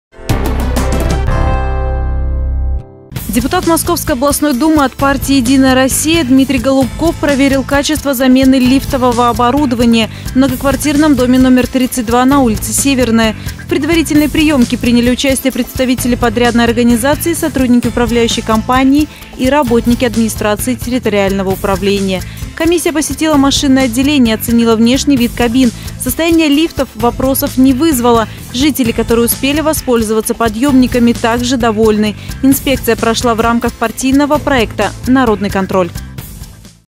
Безопасность и комфорт
Депутат Московской областной Думы от партии «Единая Россия» Дмитрий Голубков проверил качество замены лифтового оборудования в многоквартирном доме №32 на улице Северная.